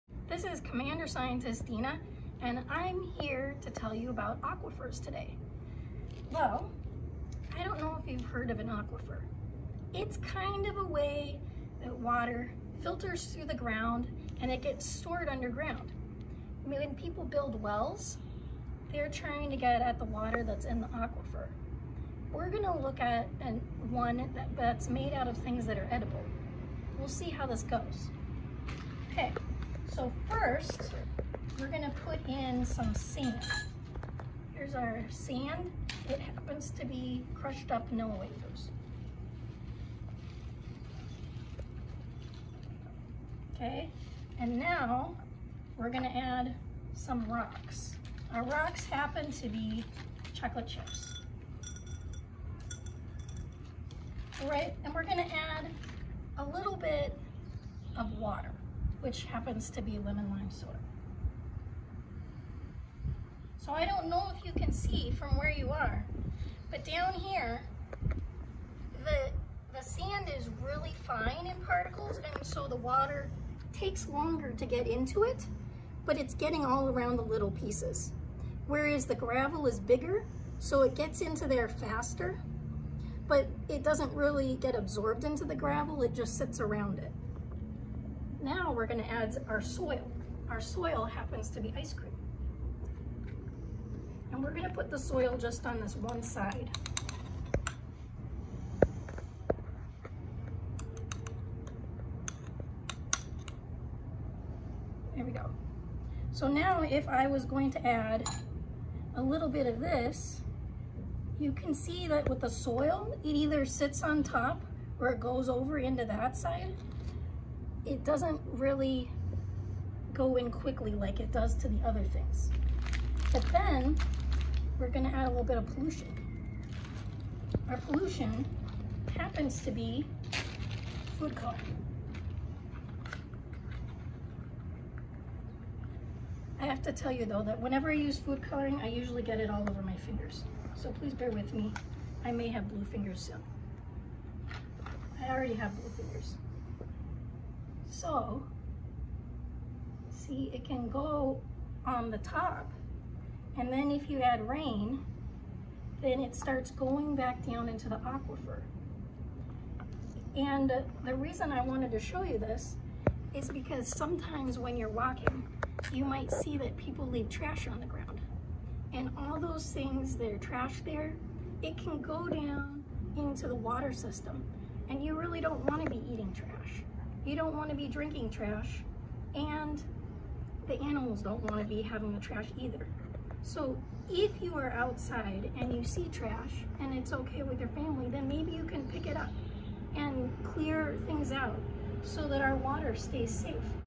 Science video 3
Resident CUMC Scientist Deena Strohman teaches us a neat experiment on aquifers! VBS day 3